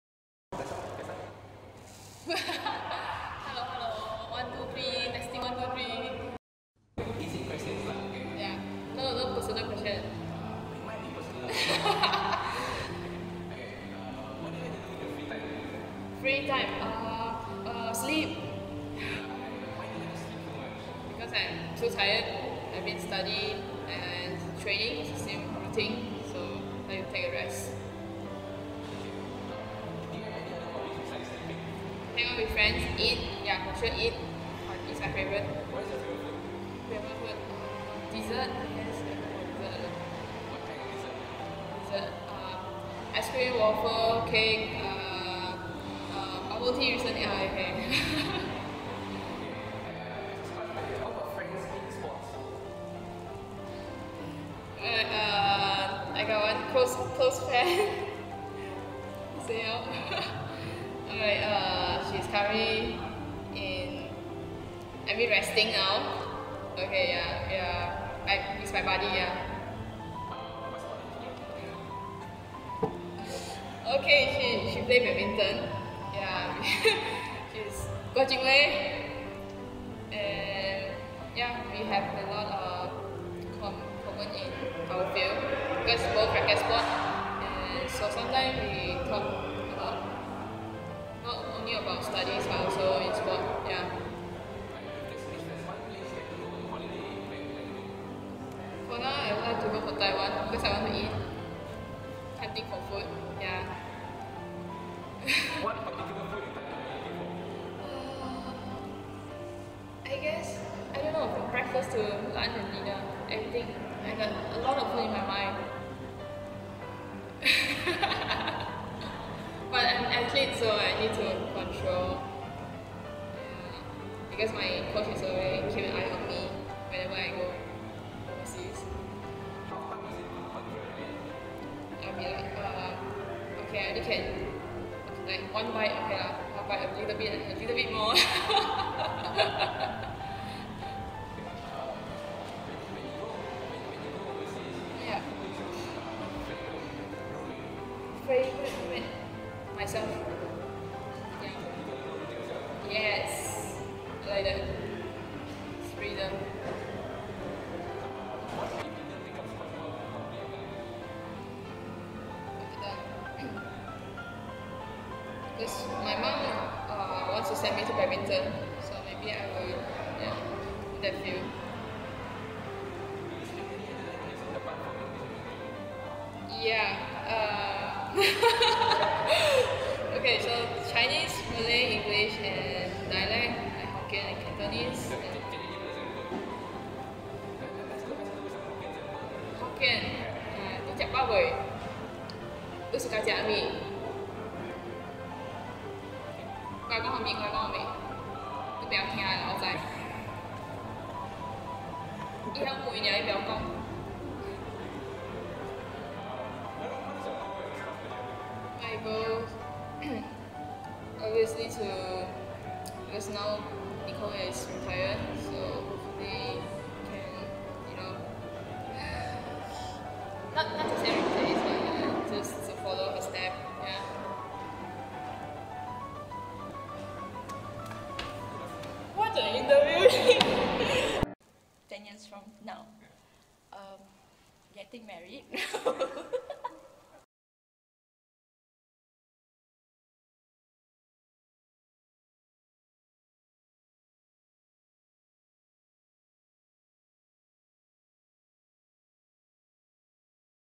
Rapid Nicks with Chan Yiwen
Find out more about Chan Yiwen - Malaysia's junior squash player in some off the court humour.

The all new segment of DLLSports - DLoL. Bringing you the lighter side of life from sports.